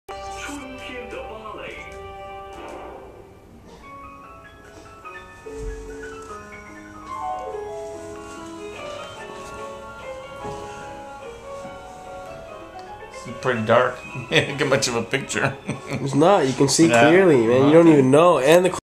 Japanese foot massage #2
Watching Shanghai TV while waiting for our japanese foot massage.